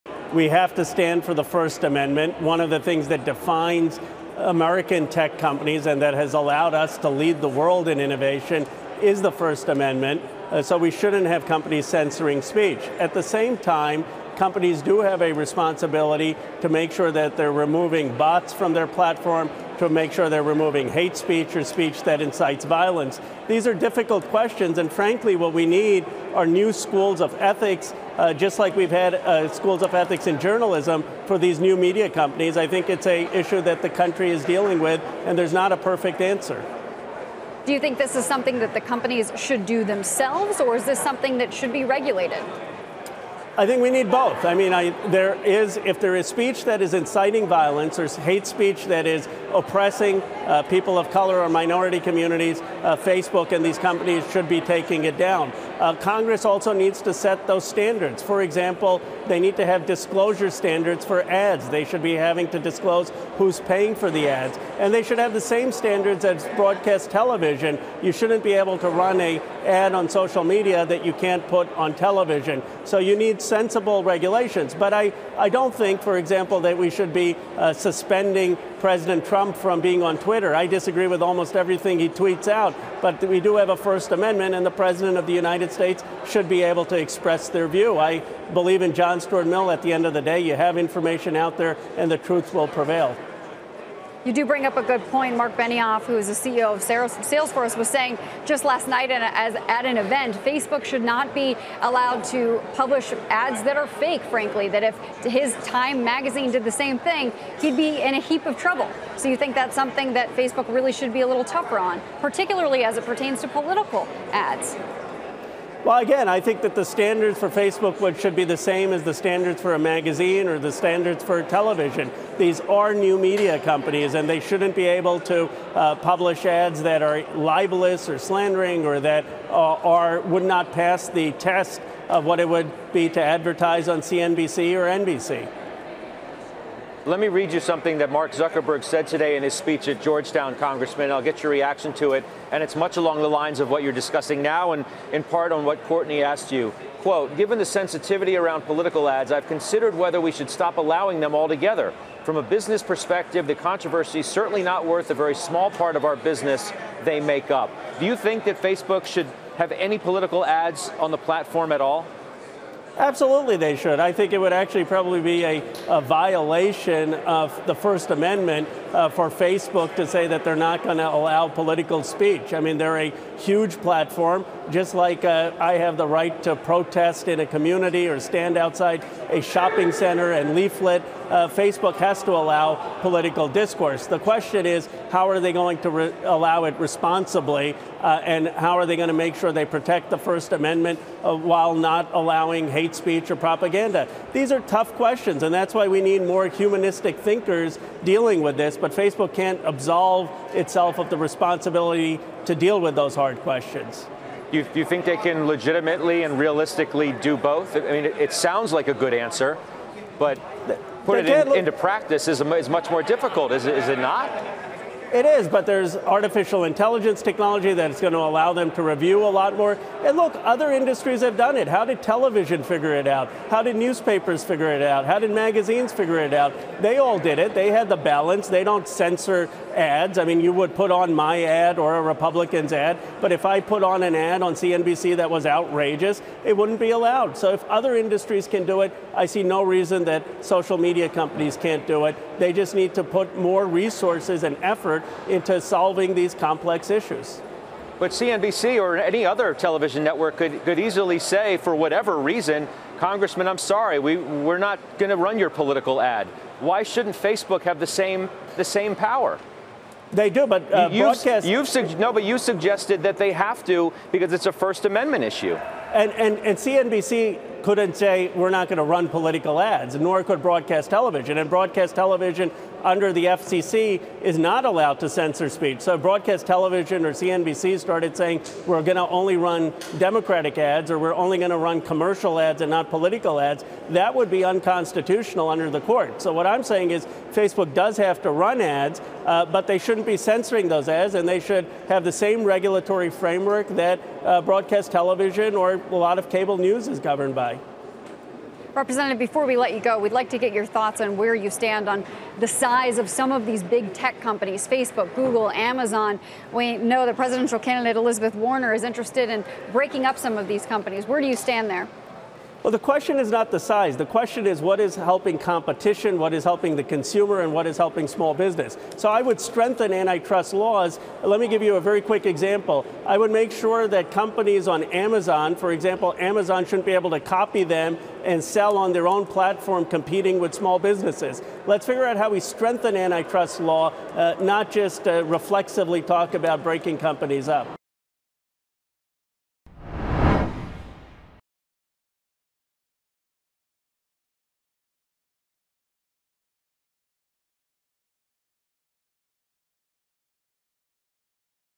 Standards for Facebook should be same as magazine or TV: Rep. Ro Khanna
Congressman Ro Khanna (D-Calif.) discusses Facebook and the growing scrutiny of Big Tech companies.